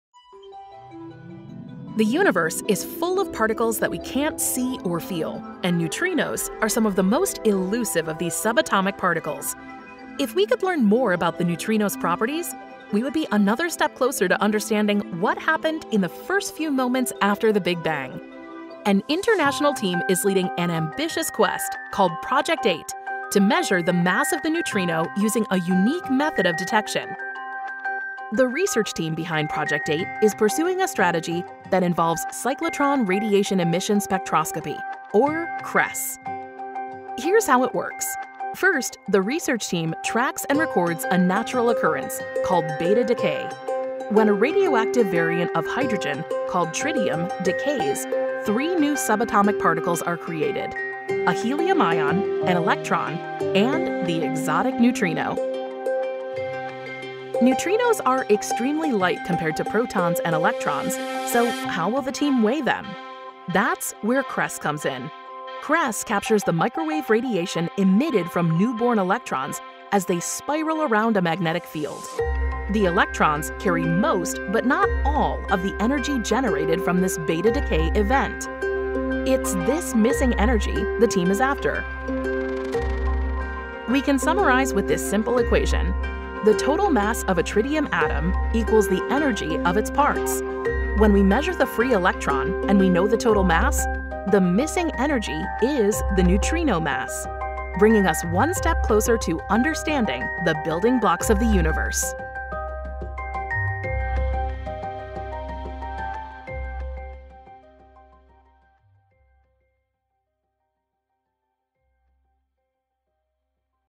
How can scientists weigh a neutrino?
An international team of researchers from the United States and Germany leading an ambitious quest called Project 8 is reporting that their distinctive strategy is a realistic contender to be the first to measure the neutrino mass. Once fully scaled up, Project 8 could help reveal how neutrinos influenced the early evolution of the universe as we know it.